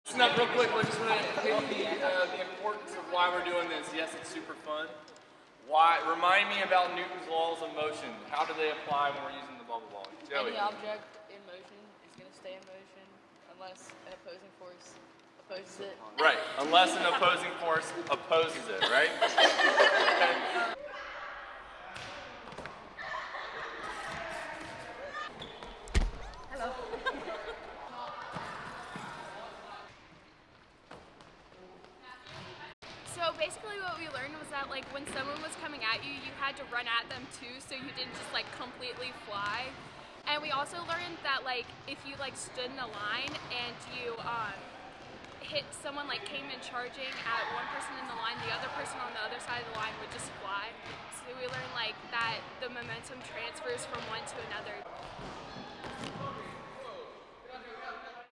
Science Bubble Balls
Villa Madonna Academy HS/Jr. High students learn science lessons from Bubble Balls.